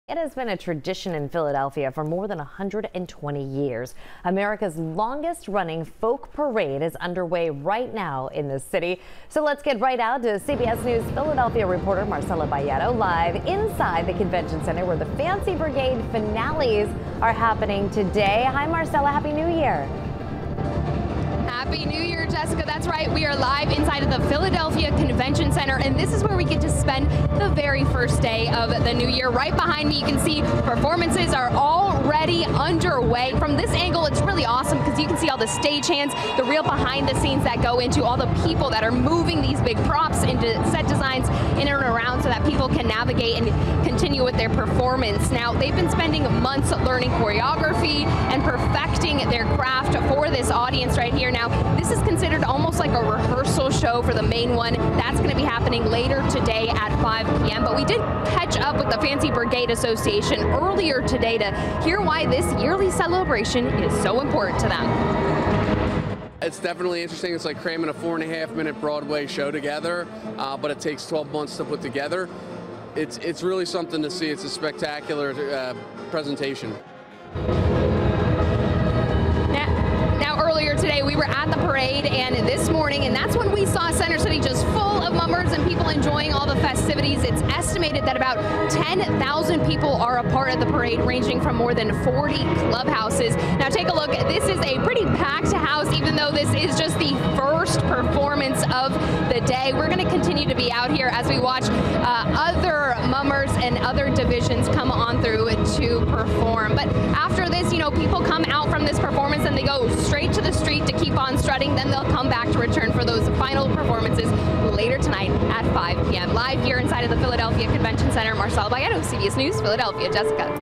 Mummers strut their stuff down Broad Street during annual New Year's Day Parade
A more than 120-year-old tradition continues on New Year's Day with Philadelphia's annual Mummers Parade.  Marcella Baietto reports from the Pennsylvania Convention Center, where crews are headed to compete in the 26th annual Fancy Brigade Finale after strutting down Broad Street in Center City.